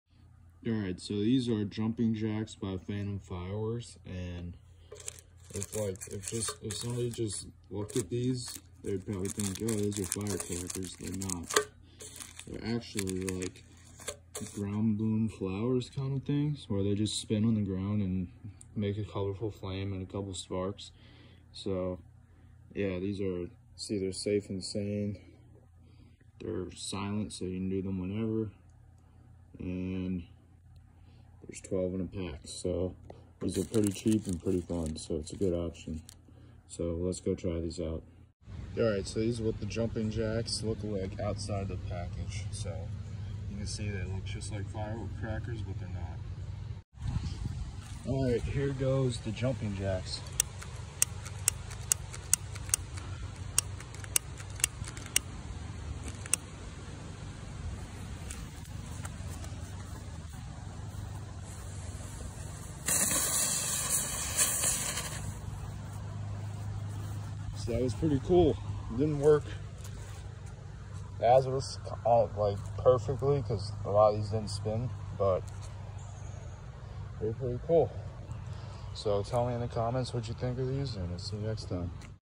Jumping Jacks by Phantom Fireworks demo (fun little novelty)
These jumping jacks are a classic firework item. They’re pretty fun to mess around with and are extremely cheap at pretty much every firework store.

I got these jumping jacks out of the Battle of Yorktown assortment, a video of me unboxing it is linked below.